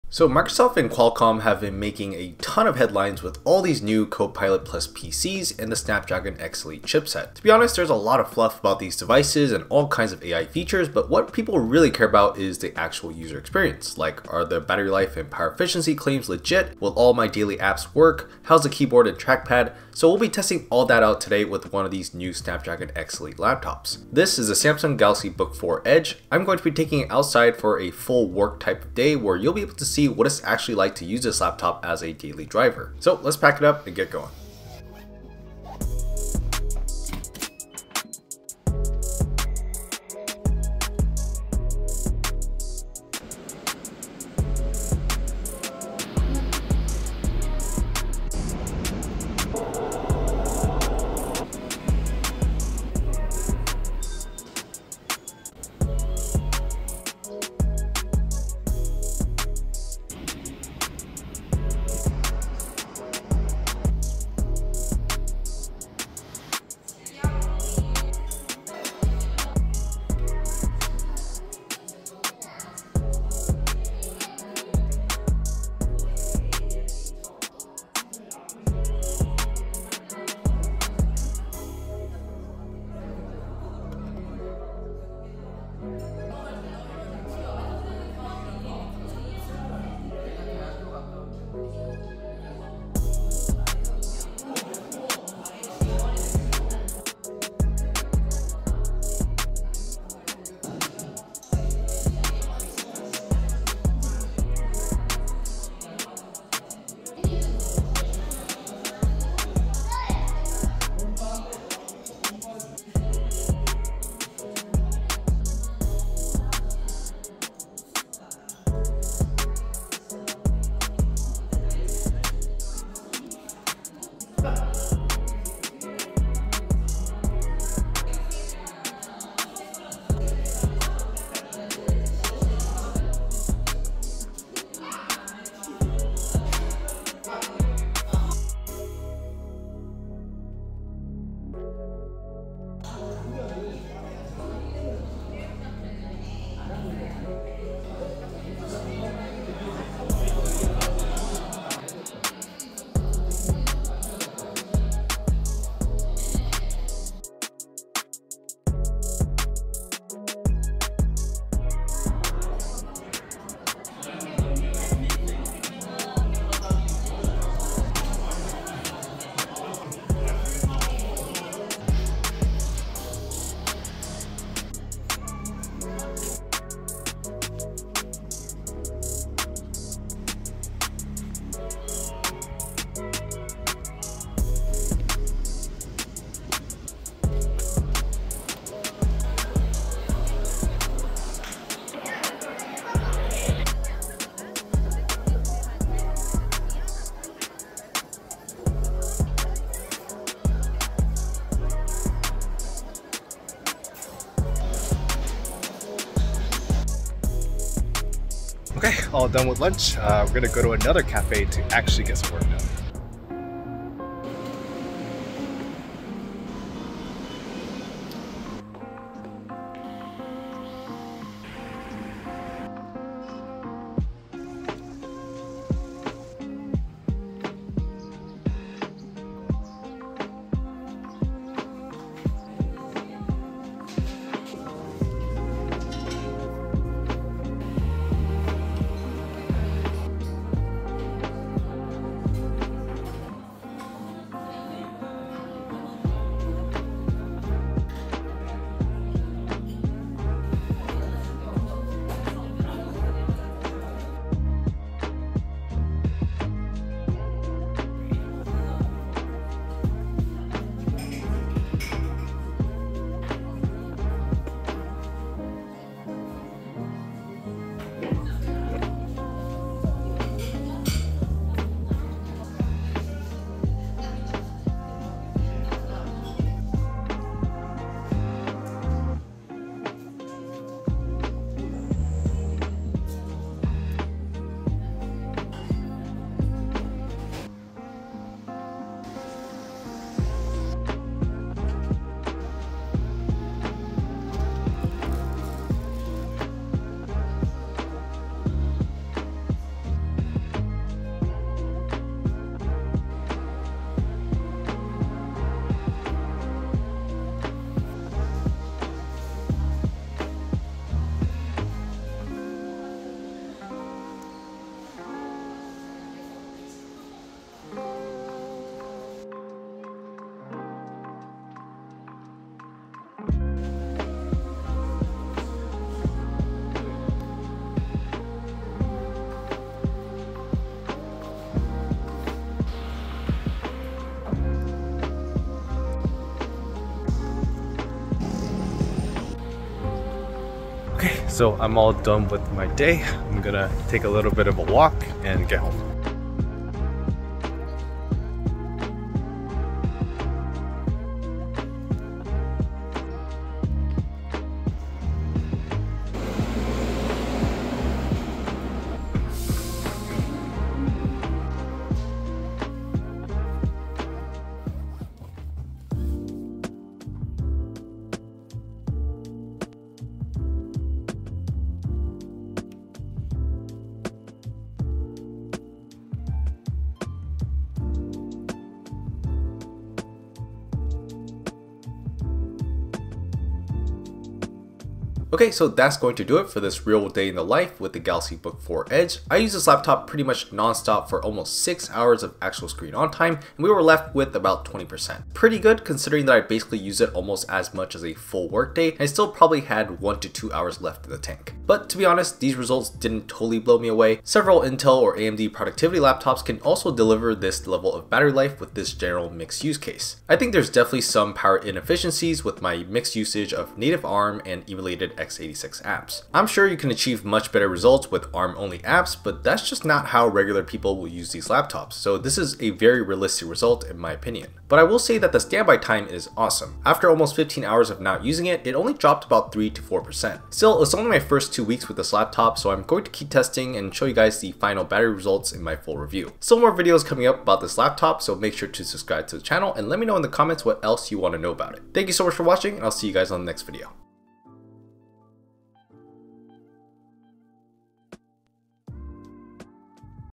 Samsung Galaxy Book 4 Edge: Real-World Battery Test with Snapdragon X Elite!
These new Copilot+ PCs like the Samsung Galaxy Book4 Edge with the Snapdragon X Elite chipset promise great power efficiency and battery life. I wanted to test that out in a real day in the life with my general usage and see if those claims are legit or not.

0:00 Context
0:34 General Usage & Productivity
2:42 Lunchtime
3:28 General Usage & Productivity
6:03 Charging Test
6:25 Results